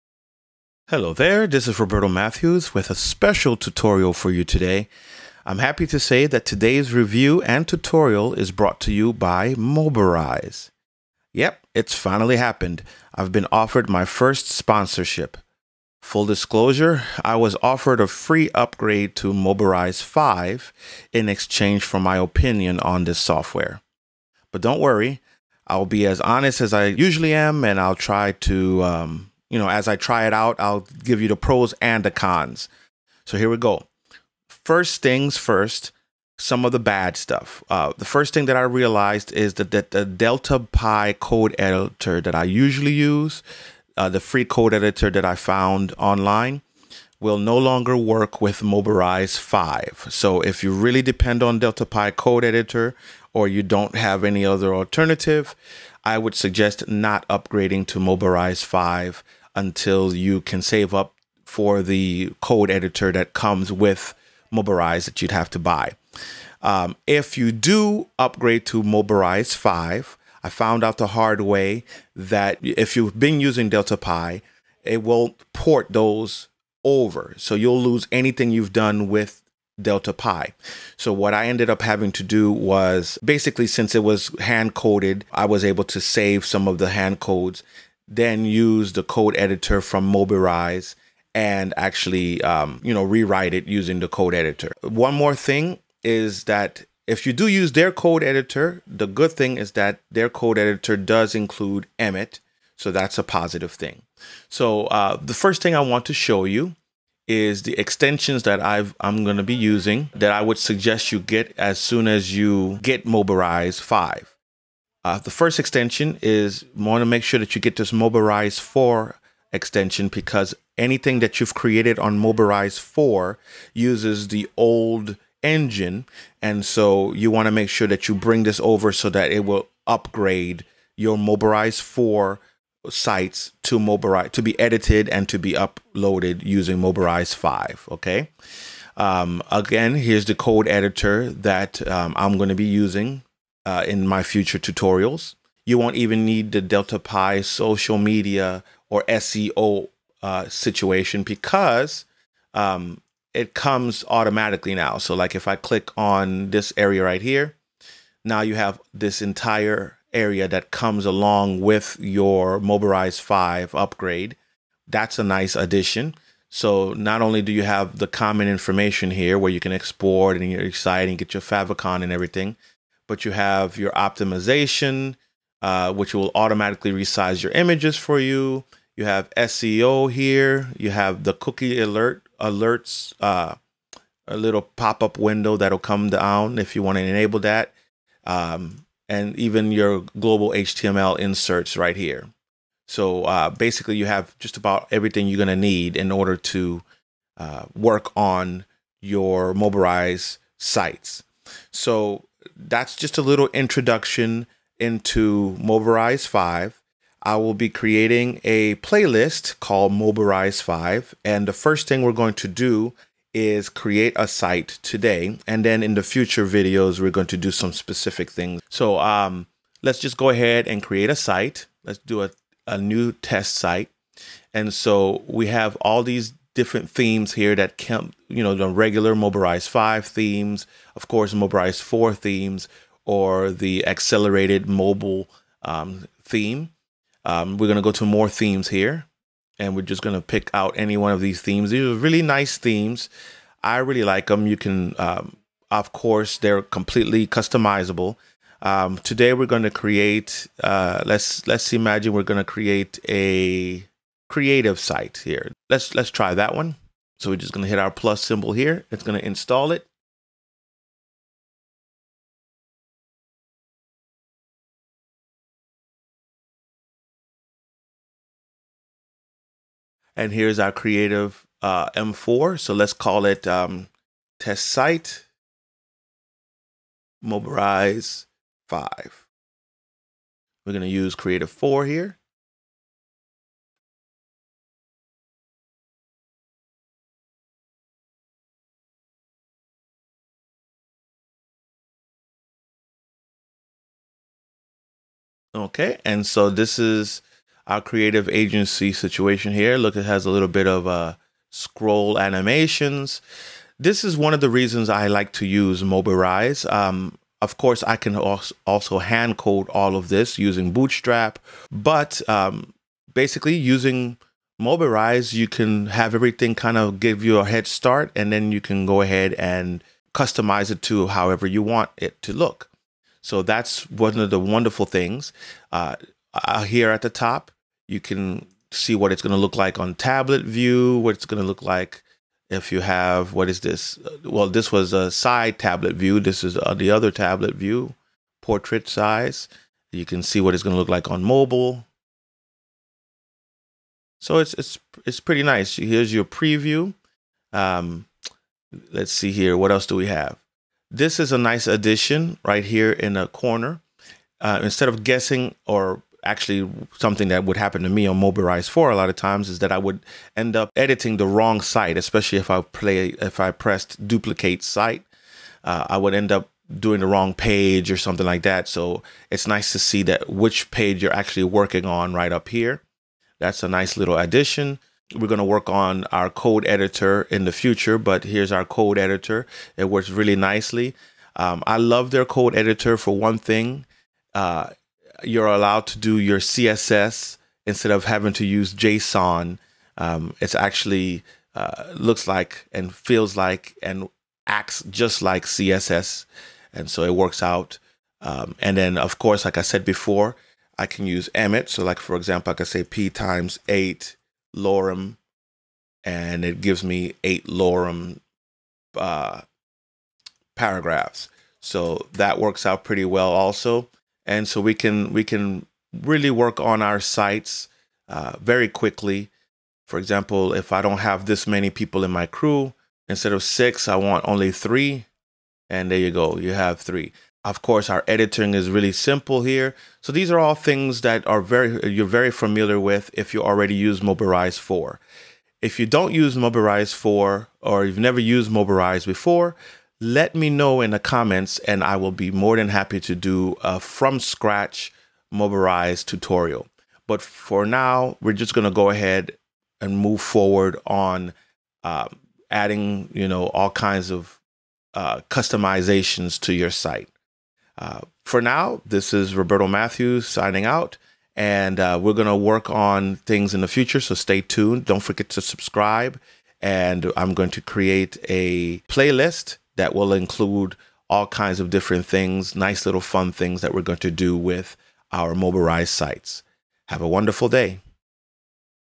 Mobirise 5 Review Intro
I have a special Mobirise 5 review tutorial for you today.  Yes! It's finally happened!  Full disclosure, I was offered a free upgrade to Mobirise 5 in exchange for my opinion.  But worry not!  I will be as honest as usual and just try it out.  So, here we go!